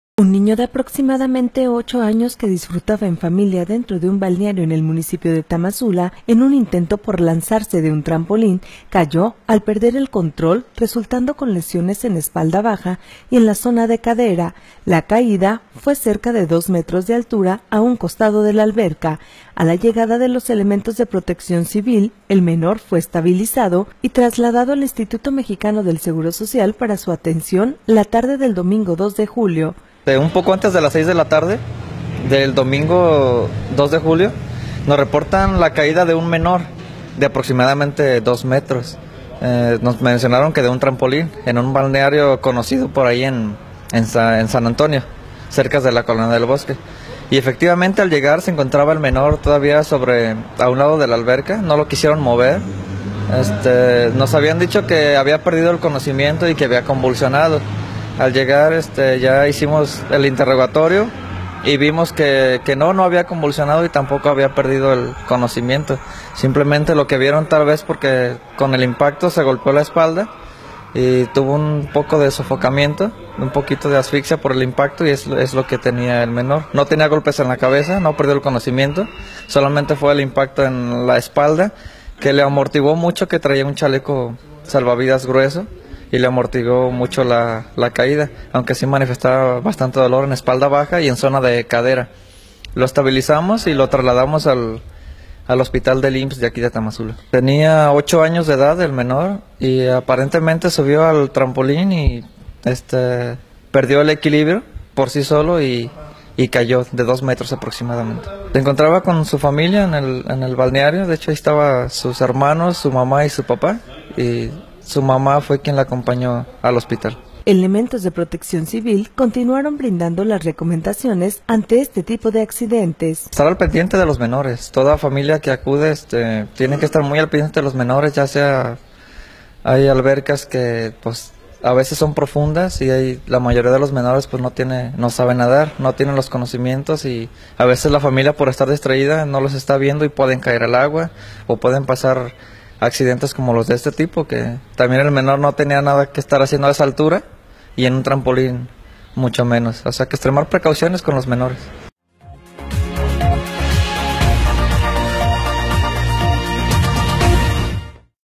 Accidente en Trampolín
Al perder el equilibrio un niño de 8 años de edad resultó con lesiones al caer de un trampolín de 2 metros de altura.